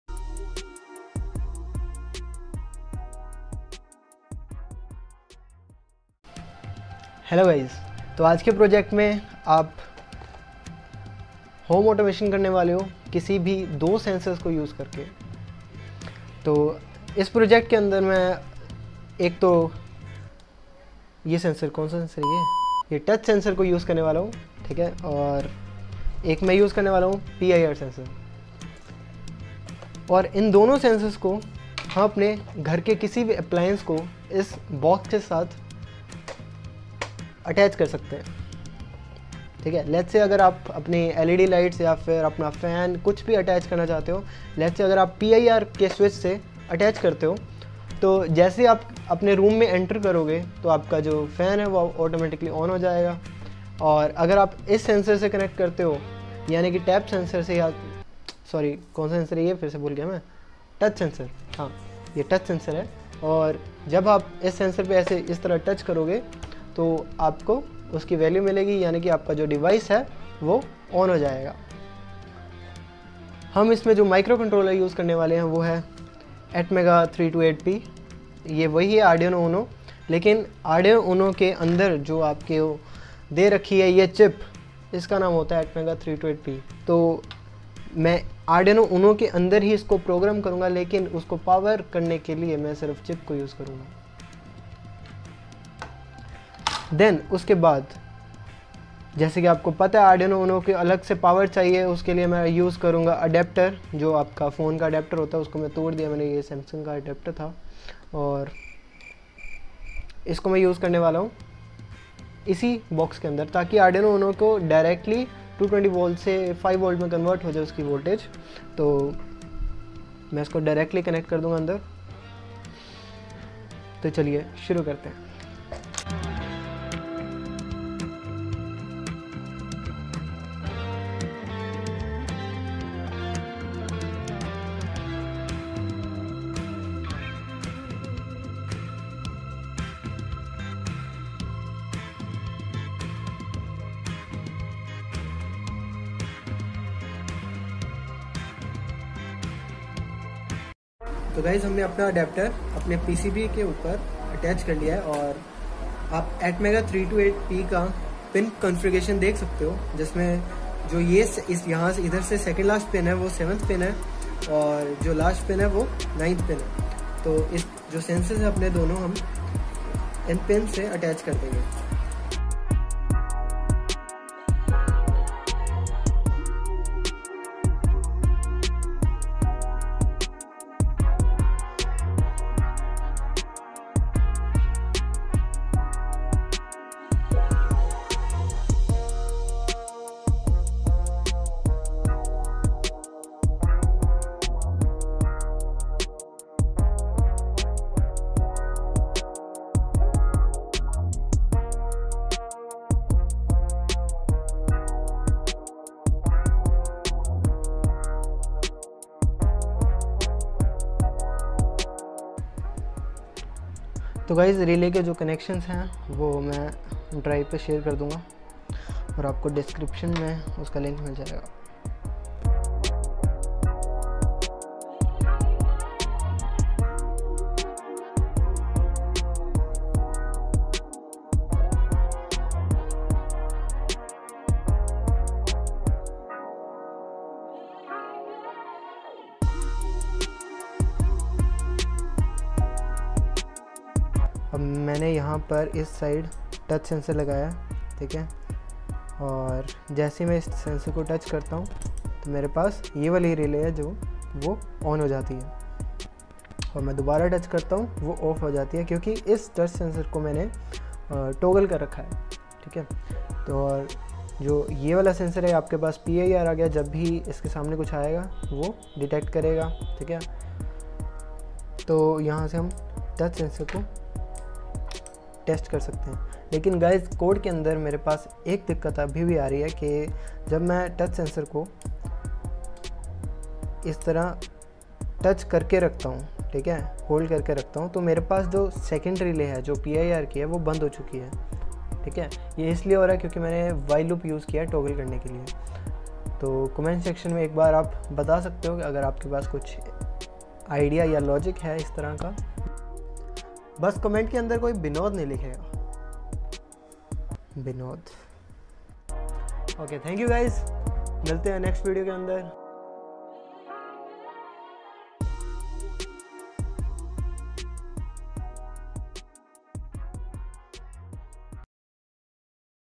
Simple Home Automation DIY box.
Touch sensor and pir sensor for controlling your home appliances.

PIR Sensor
Touch Sensor
2-channel Relay (5v)
PCB dot
ATmega328p
cap(22pf) -2
crystal oscillator (16MHz)